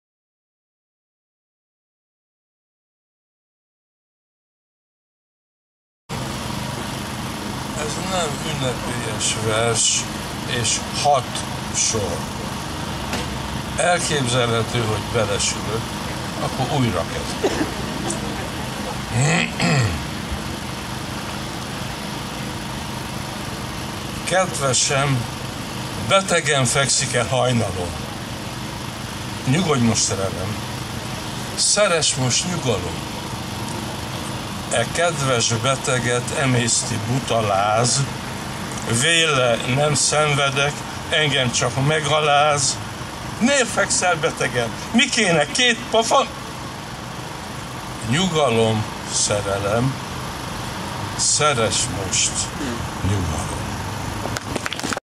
Versmaraton József Attila szobránál, Makk Károly
Szubjektív versmaraton József Attila szobránál. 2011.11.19. Szaval:
Makk Károly